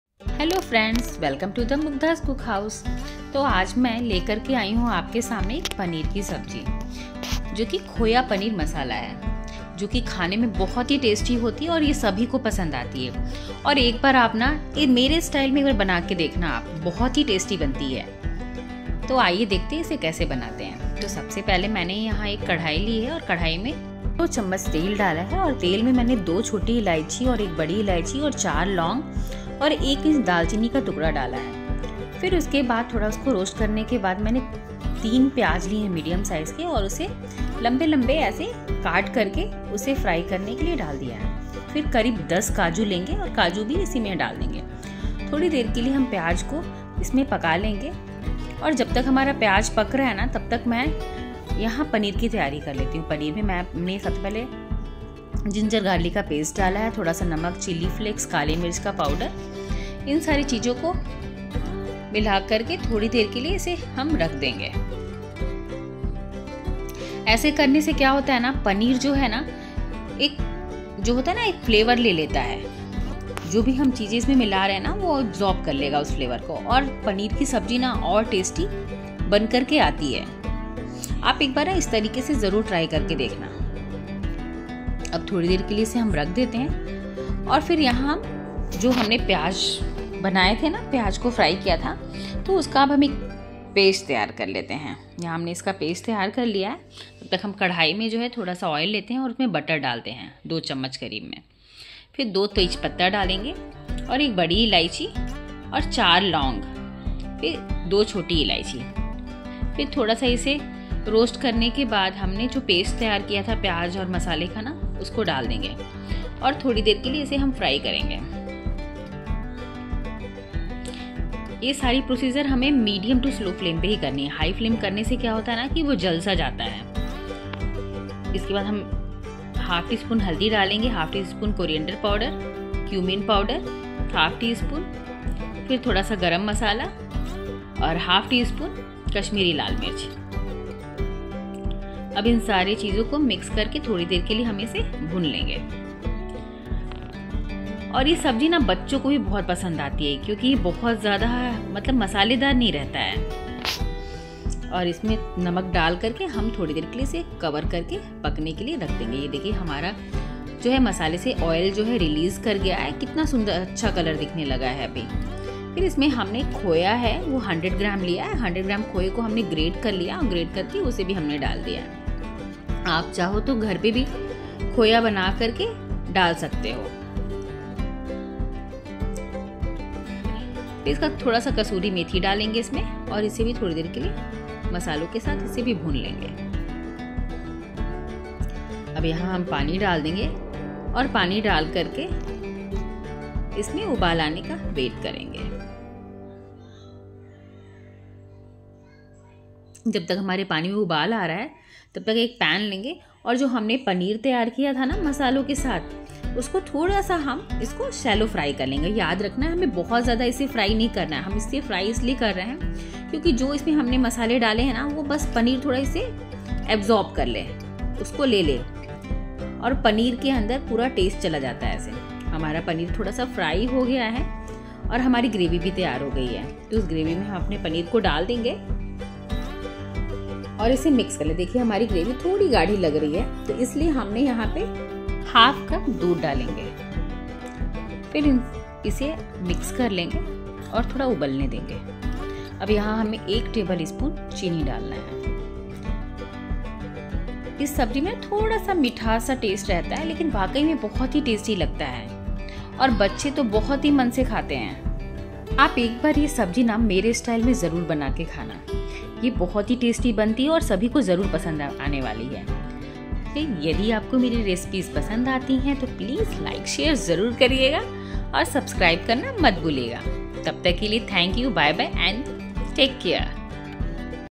खोया पनीर मसाला रेसिपी😋
Khoya paneer masala is a perfect blend of cashews, khoya, green chillies and cardamom curried with cottage cheese chunks giving a royal treat and can easily be a show stopper in any party or occasion. Serve this creamy, rich curry with some fresh home made naan or even enjoy with some zeera rice and salad. Khoya paneer is a perfect dinner option when you have guests at home.